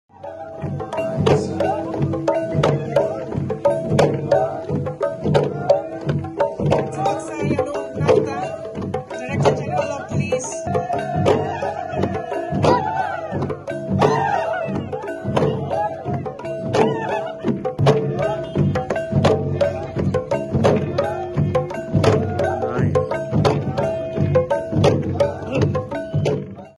Mukul joins drum beaters on 2nd day of Wangala Festival
Leader of Opposition Dr Mukul Sangma joined the traditional performers in playing the drums on the 2nd day of the Wangala Festival in Tura on Friday.